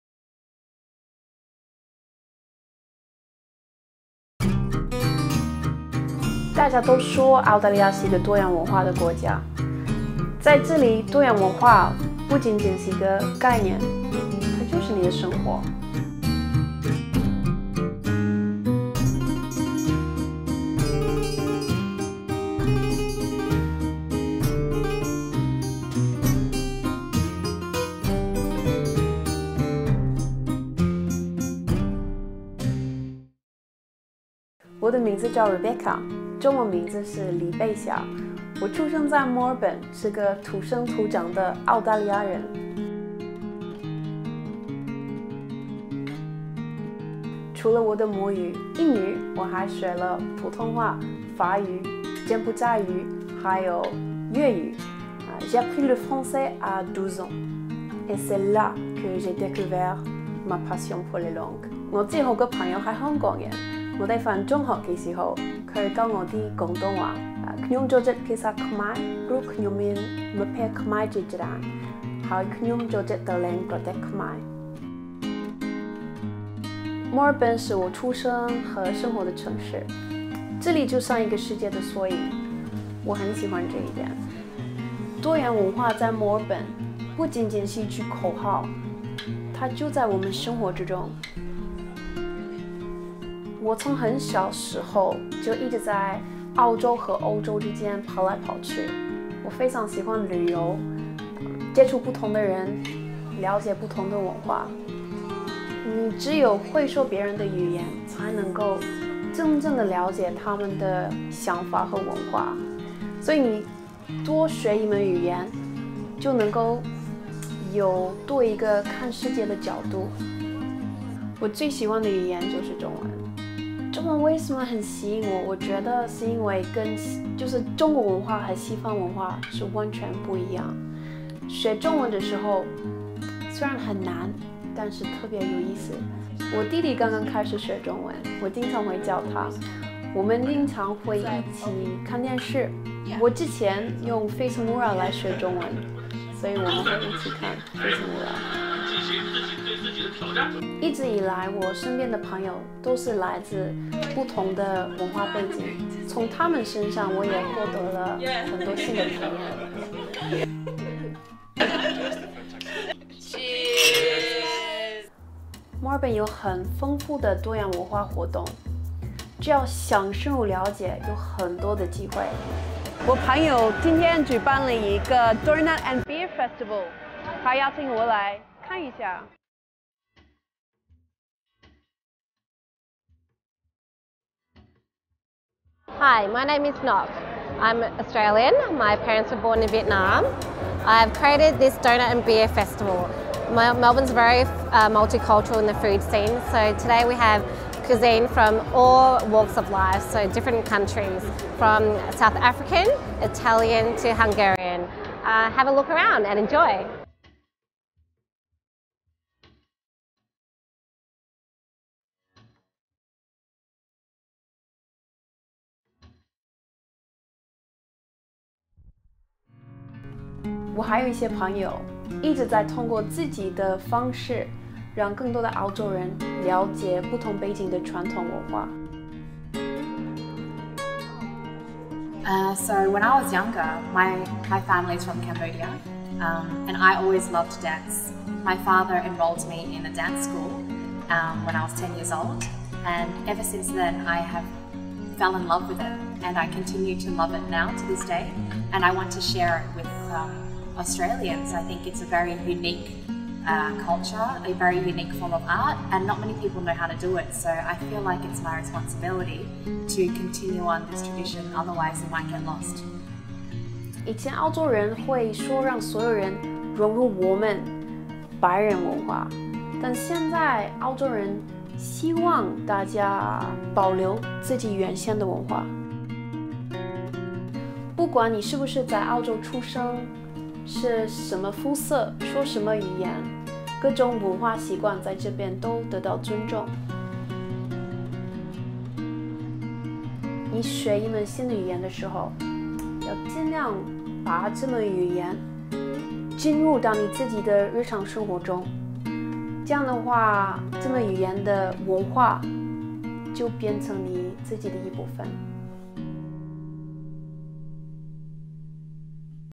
Rebecca的多元世界
Rebecca是土生土长的澳洲姑娘，80后的她已经精通六国语言，曾经为了学中文，还看了《非诚勿扰》。她说：“如果找对方法，最难的汉语其实也能学很好。”